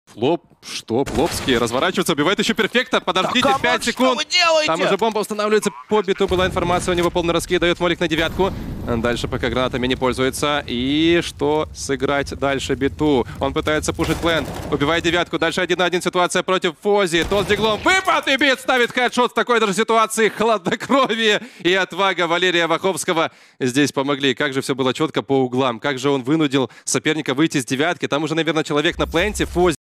бит берёт клатч 1в2 I BLAST Clips
BLAST Premier Spring Groups 2022 пройдет с 28 января по 6 февраля. Участники турнира разделят между собой 177.500$ призовых , шесть слотов на BLAST Spring Final и 6 слотов на BLAST Spring Showdown.